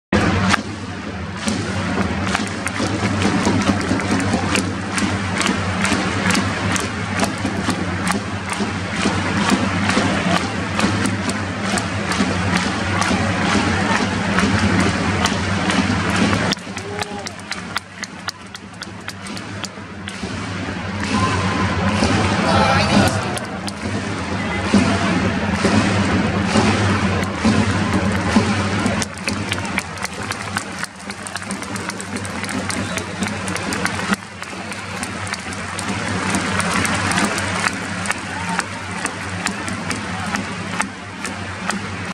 2021年10月16日 第32節 浦和レッズvsガンバ大阪 酒井のプレス
2021年10月16日 第32節 浦和レッズvsガンバ大阪
酒井が強烈なプレスをかけますがファウル、判定に納得いかない様子

Oct 16, 2021 32th sec Urawa Reds vs Gamba Osaka
"SAKAI Hiroki's pressure"